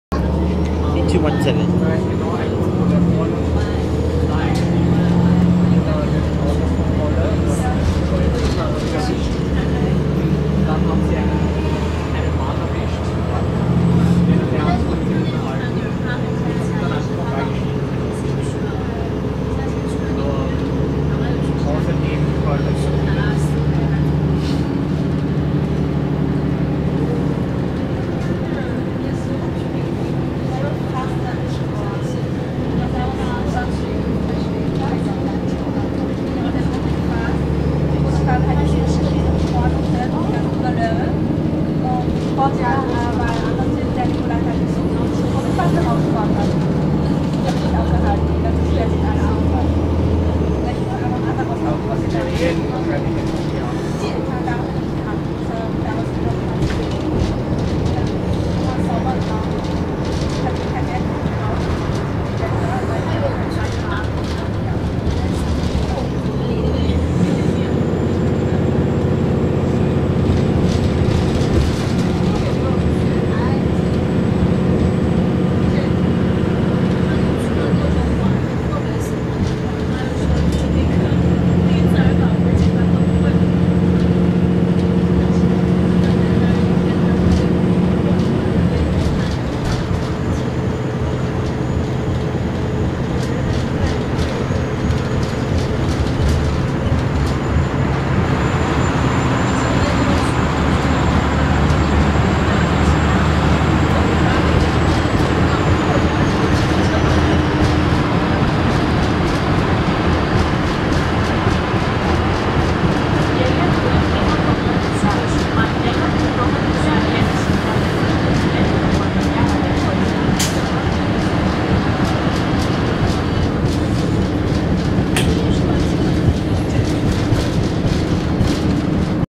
SF Muni 8217
Here is a bus ride that I took on line 28 to Geary